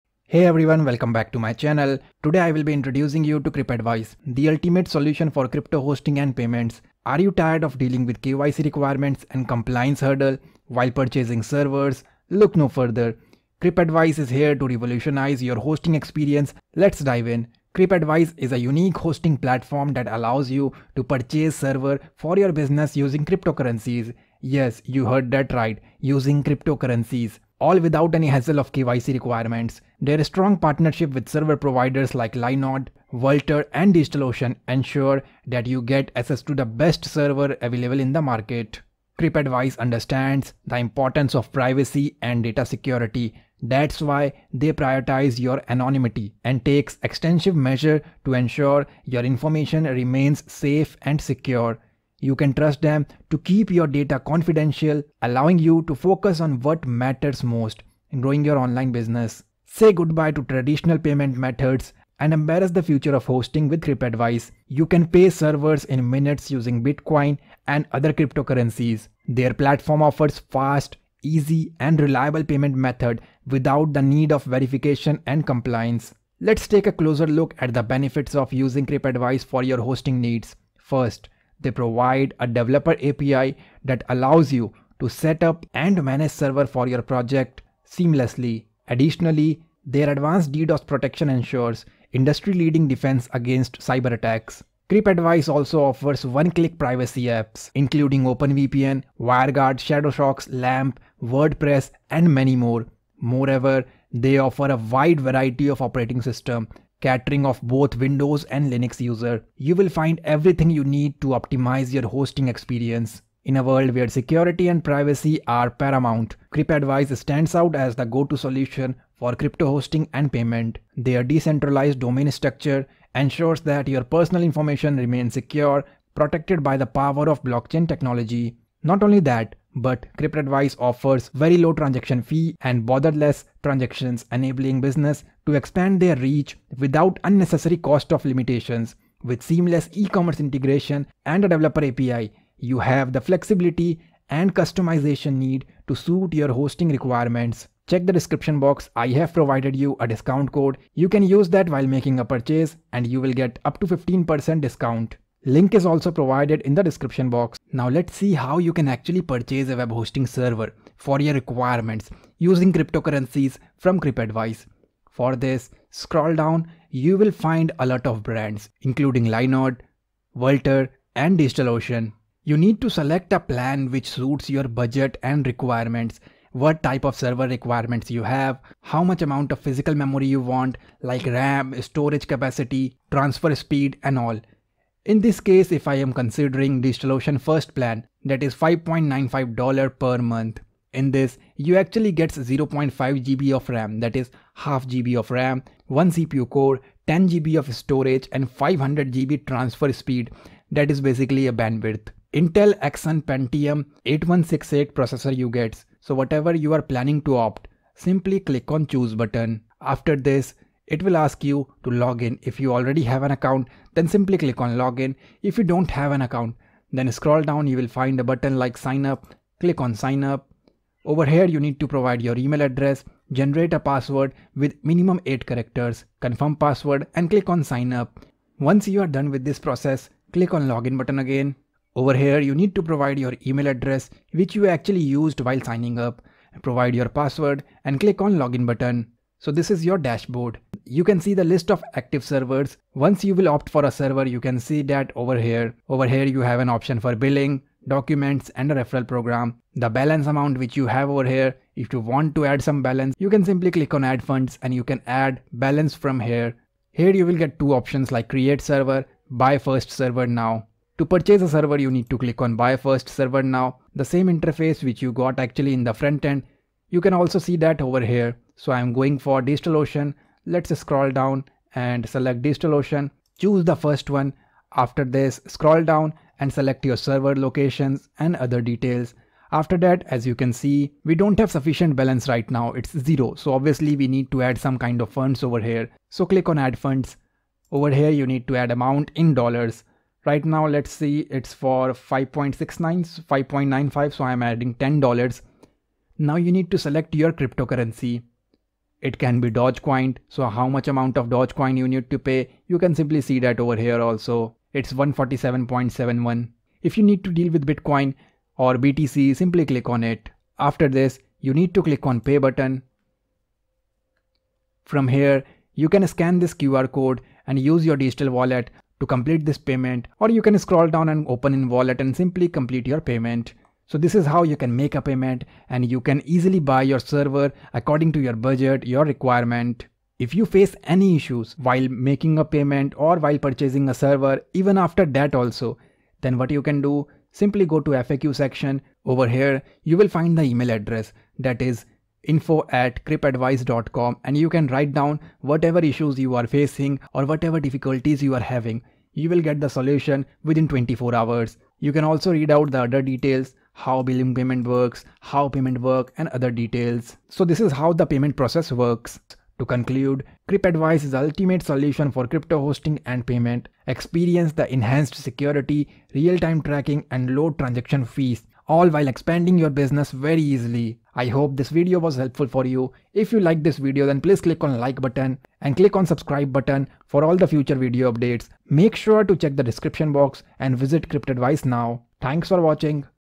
How to buy Anonymous VPS Servers using Cryptocurrency | Crypadvice Review | Best web hosting company
Get the best web hosting service for you brand and business with CrypAdvice.